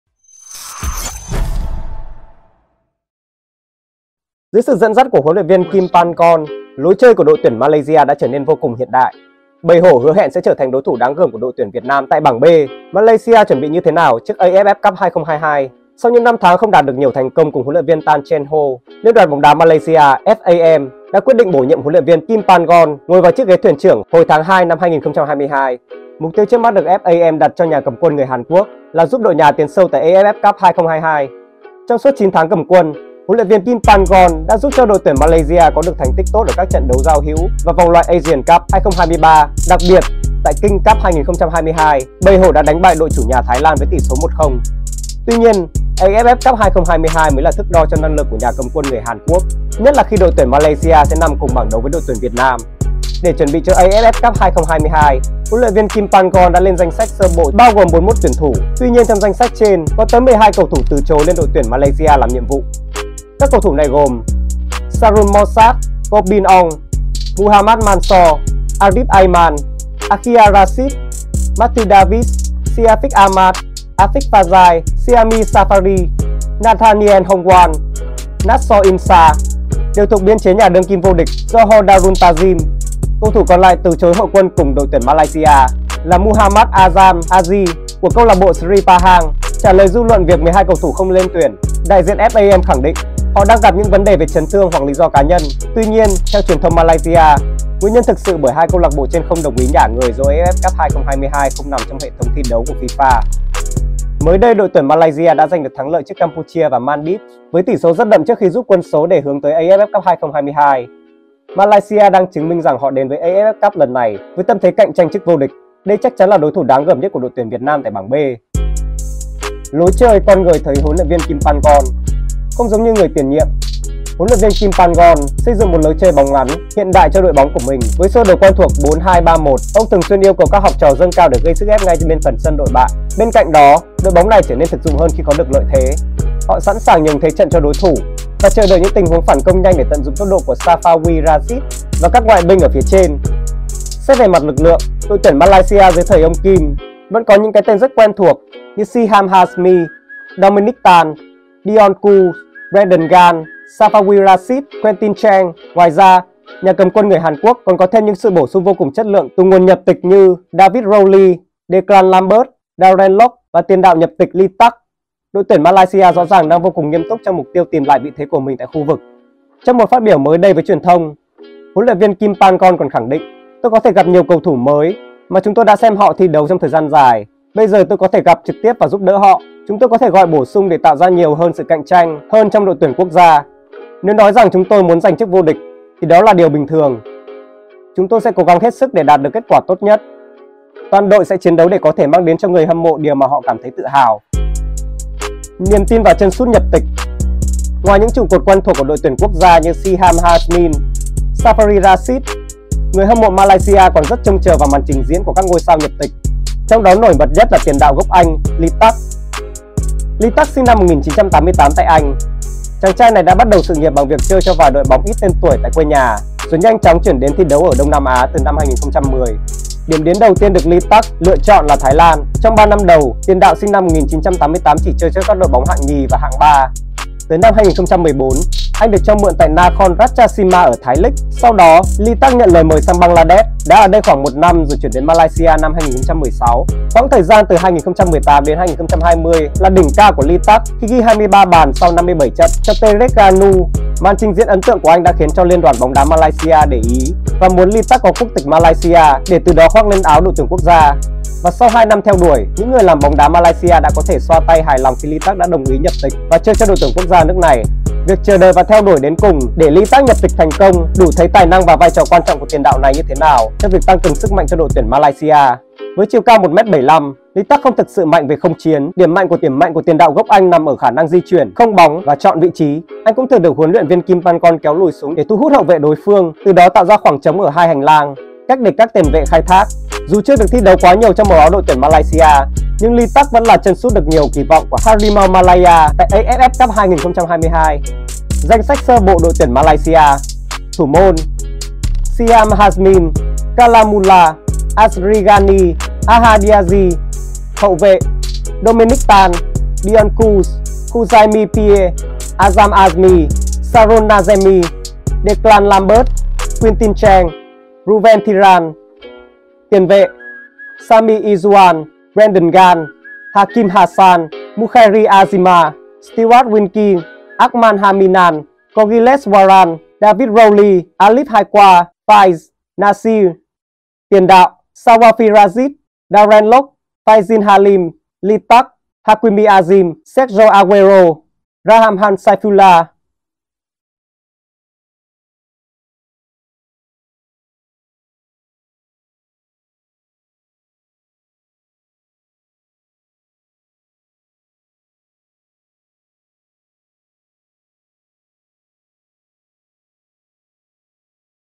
ĐT Malaysia tại AFF Cup 2022: Tiếng gầm của mãnh hổ | Cầu Thủ TV
Dưới sự dẫn dắt của HLV Kim Pan Gon, lối chơi của ĐT Malaysia đã trở nên vô cùng hiện đại. “Bầy hổ” hứa hẹn sẽ trở thành đối thủ đáng gờm của ĐT Việt Nam tại bảng B